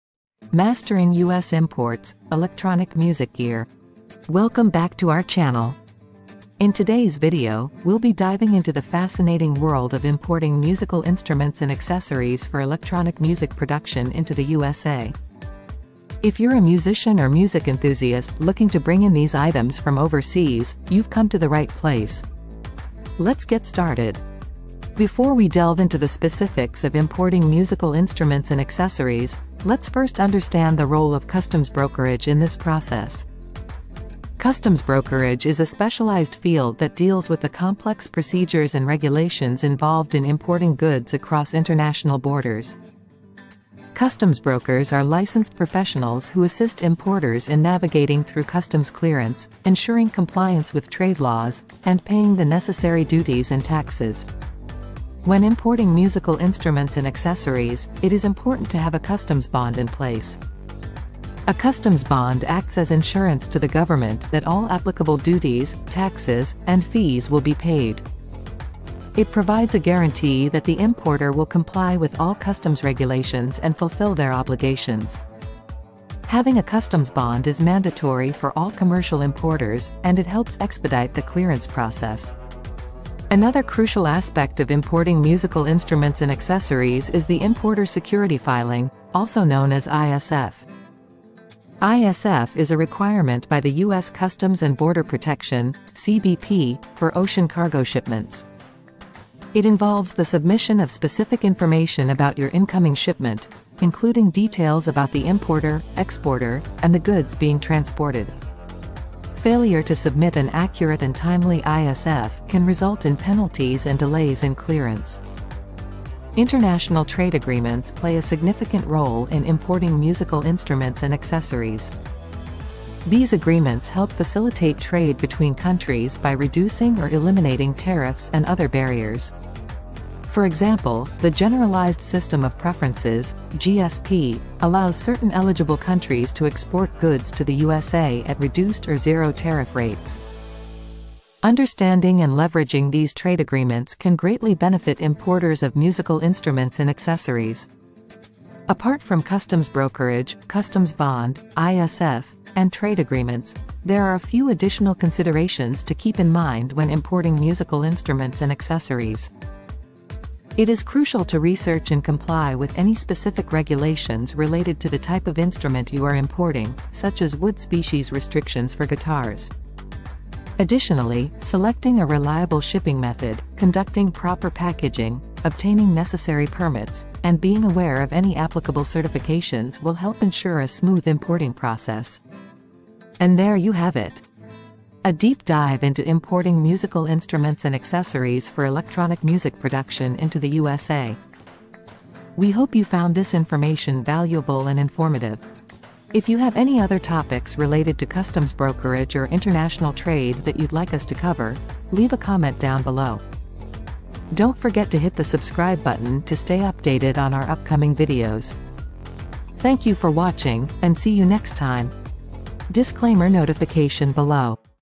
Mastering US Imports: Electronic Music Gear
ISF Customs Broker

This response provides a deep dive into the process of importing musical instruments and accessories for electronic music production into the USA. It highlights the role of customs brokerage in navigating the complex procedures and regulations involved in importing goods across international borders. The importance of having a customs bond and complying with the Importer Security Filing (ISF) requirement is emphasized. The significance of international trade agreements, such as the Generalized System of Preferences (GSP), in facilitating trade and reducing tariffs is also discussed. Additionally, it advises on researching and complying with specific regulations, selecting a reliable shipping method, conducting proper packaging, obtaining necessary permits, and being aware of applicable certifications when importing musical instruments and accessories.

1:19 - Customs bond is mandatory for all commercial importers to comply with regulations and expedite clearance process.

1:42 - Importer Security Filing (ISF) is required by US Customs for ocean cargo shipments, failure to submit accurate ISF can result in penalties and delays.

2:07 - International trade agreements help reduce tariffs and facilitate trade for importing musical instruments and accessories.

2:50 - Research and comply with specific regulations for the type of instrument being imported, select reliable shipping method, proper packaging, permits, and certifications are important for a smooth importing process.

3:12 - Information provided on importing musical instruments and accessories for electronic music production into the USA, suggestions for future topics related to customs brokerage and international trade are welcome.